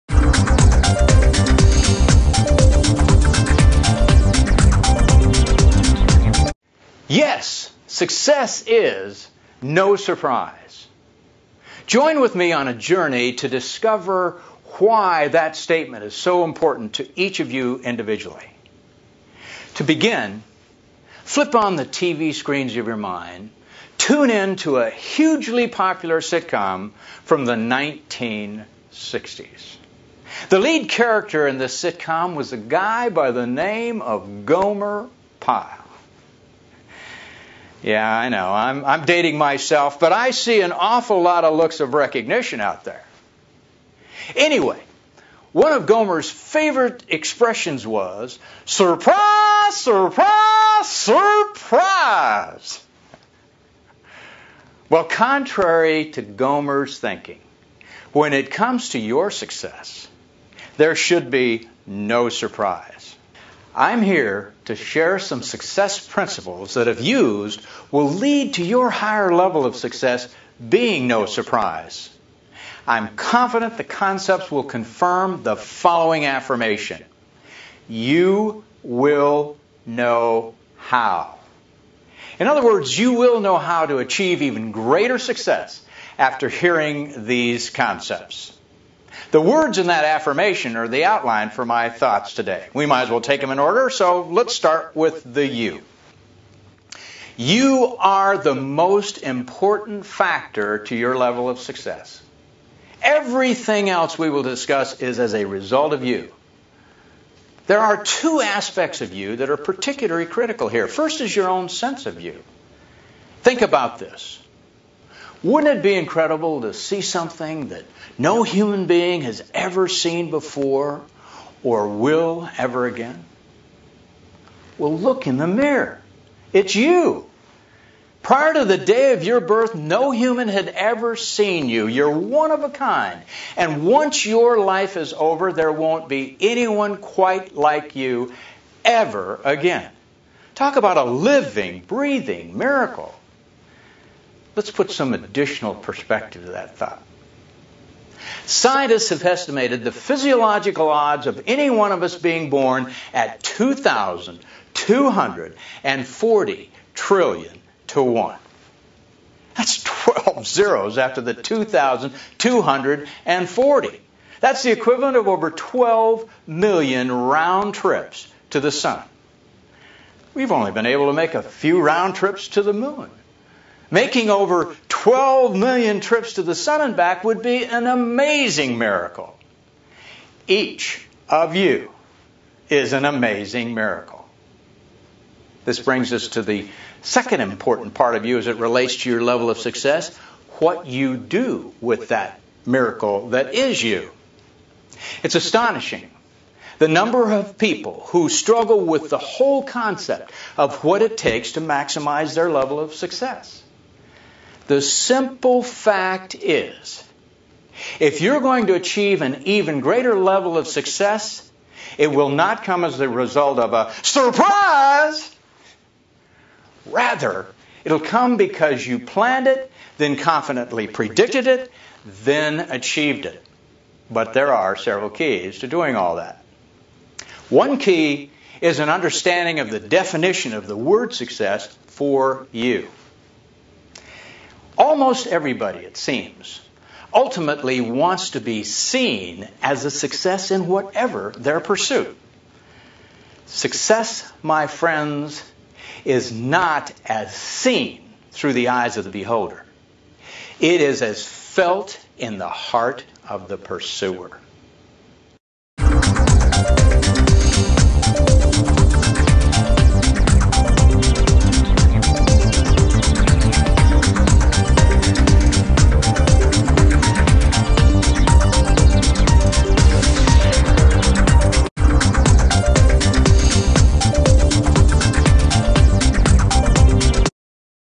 Success is No Surprise: Four Keys to Success
Excerpt from Gary Greenfield's popular workshop and keynote presentation entitled, "Success is No Surprise". In experiencing this workshop/keynote you are provided tactics and understanding of the critical keys to success to help drive greater personal growth, productivity and profit. Find out what Gary has to offer your company that will help you "Profit Through Performance."